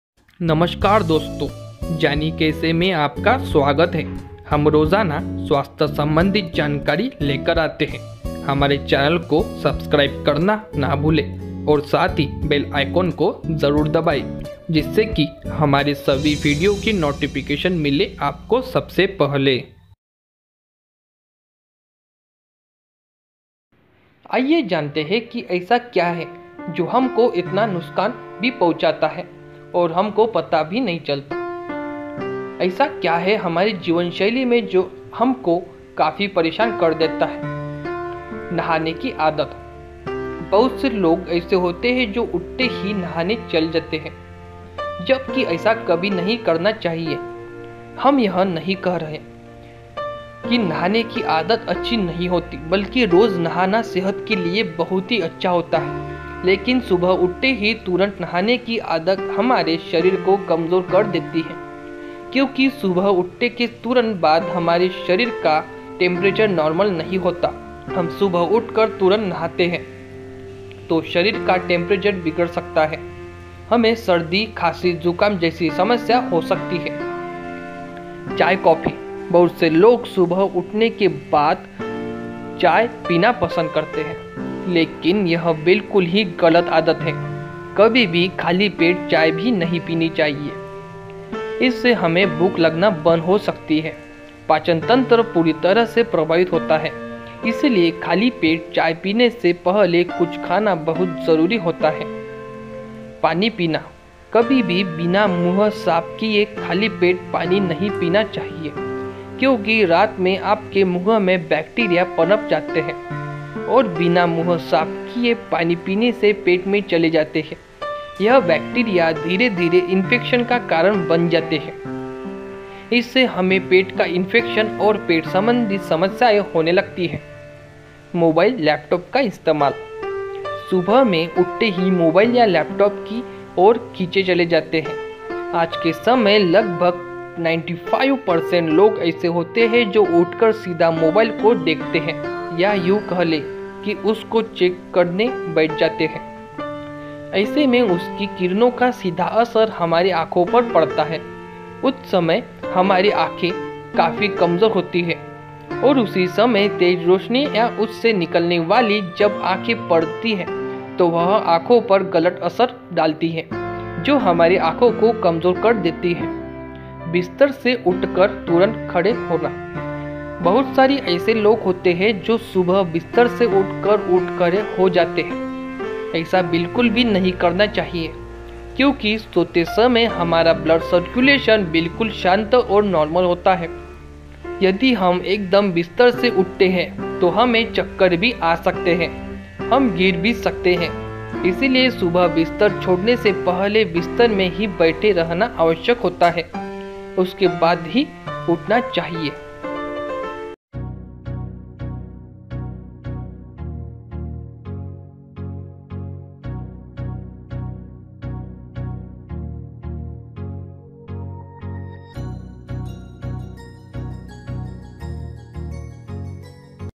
बहुत कमजोर हो जाता है उन लोगों का शरीर जो सुबह उठकर सबसे पहले करते है ये 5 काम, आप भी तो नही शामिल…
आजकल की भागदौड़ में हम कई ऐसी गलतिया कर बैठते हैं जिसकी वजह से हमको बुरे परिणाम भुगतने पड़ते हैं । जब हम ऐसा कुछ करते हैं तो हमको उस समय तो इन बातों का पता नहीं लगता अपर एक उम्र के बाद हमको अहसास होने लगता है क्योंकि वक़्त से पहले ही हमारा शरीर थक जाता है । कमजोर हो जाता है उन लोगों का शरीर, जो सुबह उठकर सबसे पहले करते है ये 5 कामआज के अपने इस पोस्ट के माध्यम से हम उनही गलतियों के बारे में बात करेंगे जो आपको बताएँगे की आप कहा और क्या गलतियाँ कर रहे हो । ऐसा नहीं है की हम जानबूझकर ऐसा कर रहे हैं आज कल की जीवन शैली हो ही ऐसी गई है की जिसकी वजह से हम ऐसे रह रहे हैं और अनजाने में गलतियाँ कर बैठते हैं ।आइये जानते हैं की ऐसा क्या है जो हमको इतना नुकसान भी पहुंचा जाता है और हमको पता तक नहीं चलता । ऐसा क्या है हमारी जीवन शैली में जो हमको काफी परेशान कर देता है । आइये जानते ऐन उन आदतों के बारे में ।

नहाने की आदत

बहुत से लोग ऐसे होते हैं जो उठते ही नहाने चल देते हैं । जब की ऐसा कभी भी नहीं करना चाहिए । हम यह नहीं कह रहे हैं की नहाने की आदत अच्छी नहीं होती बल्कि रोज नहाना सेहत के लिए बहुत ही अच्छा होता है लेकिन सुबह उठते ही तुरंत नहाने की आदत हमरे शरीर को कमजोर कर देती है क्योंकि सुबह उठने के तुरंत बाद हमारे शरीर का टेंपरेचर नॉर्मल नहीं होता है। हम सुबह उठकर तुरंत नहाते हैं तो शरीर का टेंपरेचर बिगड़ सकता है। हमें सर्दी-खांसी-जुकाम जैसी समस्याएं हो सकती हैं।

चाय कॉफी

बहुत से लोग सुबह उठने के तुरंत बाद चाय पीना पसंद करते हैं । लेकिन यह बिल्कुल ही गलत आदत है, कभी भी खाली पेट चाय नहीं पीनी चाहिए। इससे हमे भूख लगना बंद हो जाती है और पाचन तंत्र पूरी तरह से प्रभावित होता है। इसलिए खाली पेट चाय पीने से पहले कुछ खाना बहुत जरूरी होता है।

पानी पीना

कभी भी बिना मुंह साफ किए खाली पेट पानी नहीं पीना चाहिए। क्योंकि रात में आपके मुंह में बैक्टीरिया पनप जाते हैं और बिना मुंह साफ किए पानी पीने से पेट में चले जाते हैं। यह बैक्टीरिया धीरे-धीरे इन्फेक्शन का कारण बन जाते हैं। जिससे हमें पेट का इंफेक्शन और पेट संबंधी समस्याएं होने लगती हैं।

मोबाइल लैपटॉप का इस्तेमाल

सुबह में उठते ही मोबाइल या लैपटाप की और खिचें चले जाना । आज के समय में लगभग 95% लोग ऐसे हैं जो उठकर सीधा मोबाइल को देखते हैं या यूं कह ले की उसको चेक करने बैठ जाते है । ऐसे में उसकी किरणों का सीधा असर हमारी आँखों पर पड़ता है । उस समय हमारी आंखे काफी कमजोर होती है और उसी समय में तेज़ रोशनी या उससे निकली किरने जब आंखो पर पड़ती है तो वह आंखो पर गलत असर डालती है जो हमारी आंखो को कमजोर कर देती है ।

बिस्तर से उठकर तुरंत खड़े होना

बहुत सारे ऐसे लोग होते हैं, जो सुबह बिस्तर से तुरंत उठ कर खड़े हो जाते हैं। ऐसा बिल्कुल भी नहीं करना चाहिए, क्योंकि सोते समय हमारा ब्लड सरकुलेशन बिल्कुल शांत और नार्मल होता है। यदि हम एकदम बिस्तर से उठते हैं तो हमें चक्कर भी आ सकते हैं। हम गिर भी सकते हैं। इसलिए सुबह बिस्तर छोड़ने से पहले बिस्तर में ही बैठे रहना आवश्यक होता है। उसके बाद ही उठना चाहिए